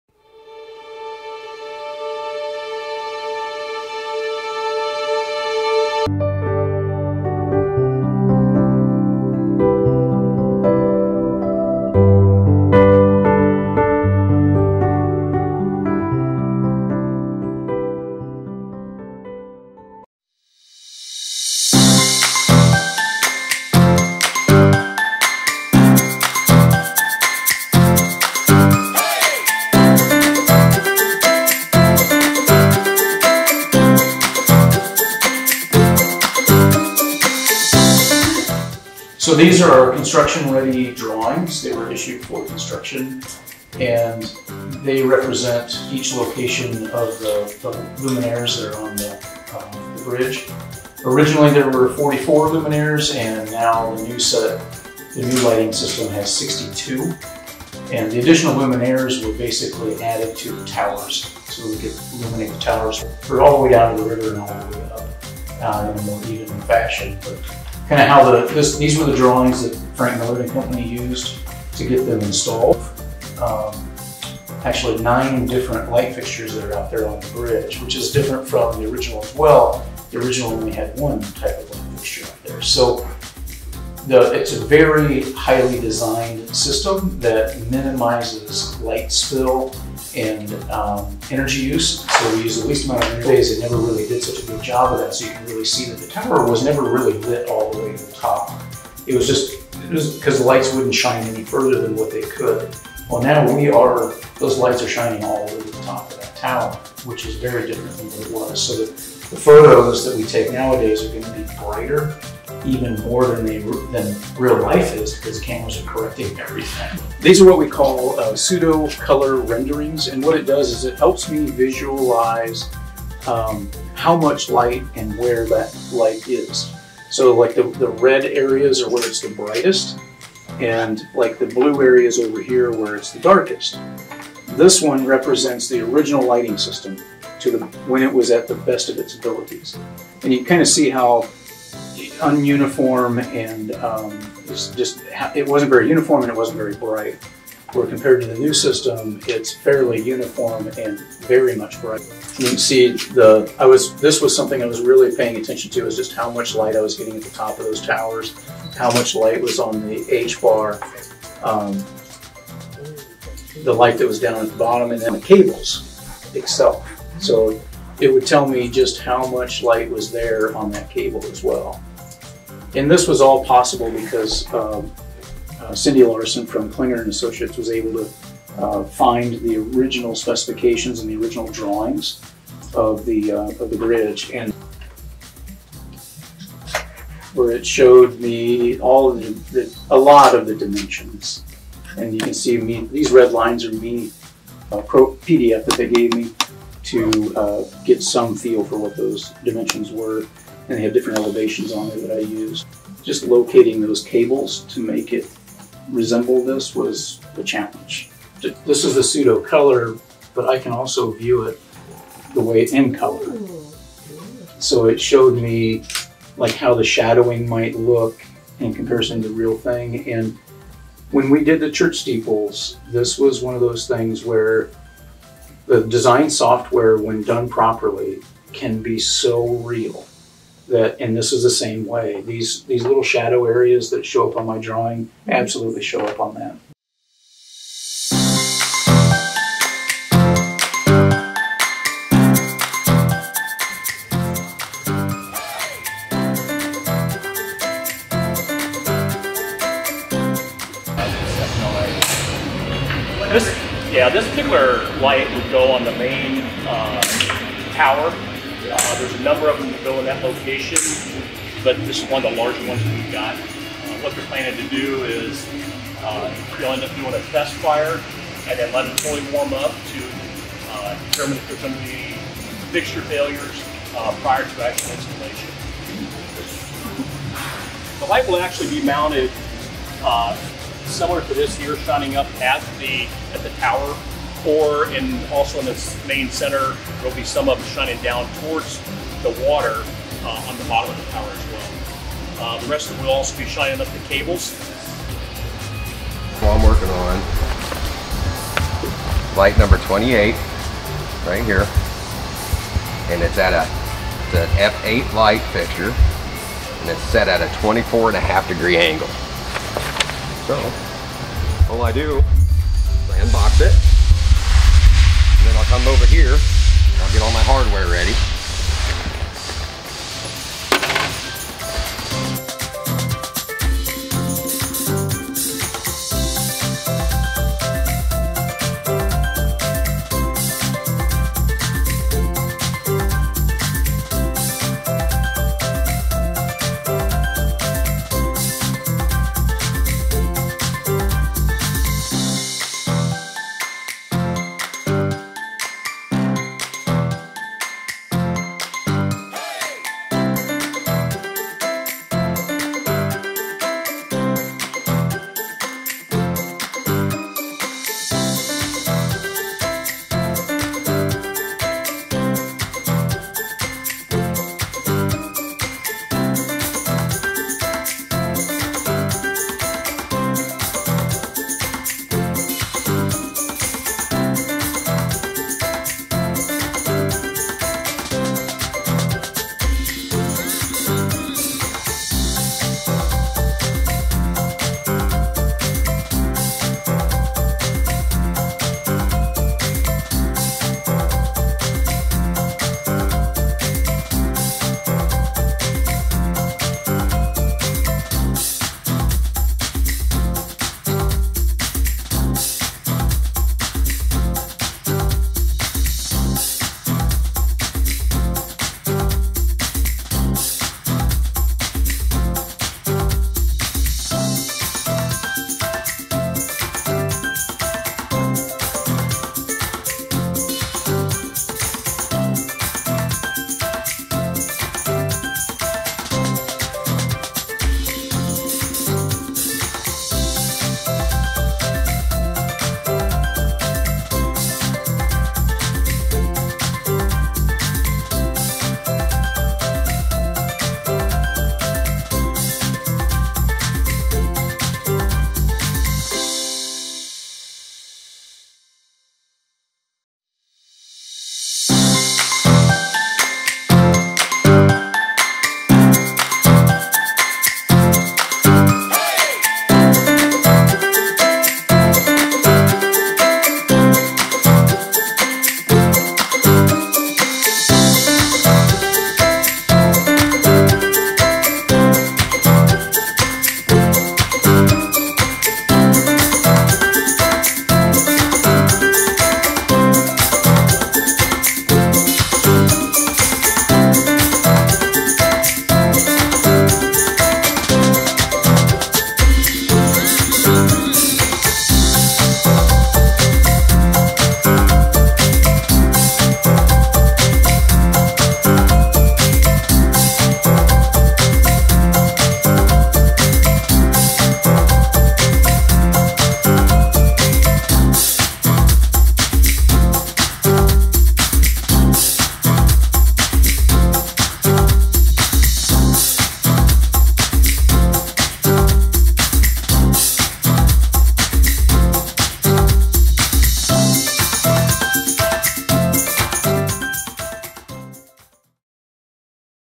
Lightscape Great River Bridge Lighting Documentary
The Lightscape project in Burlington, IA illuminates the Great River Bridge with the help of local donations and a determined committee.  This video documents the work and shows how they did it!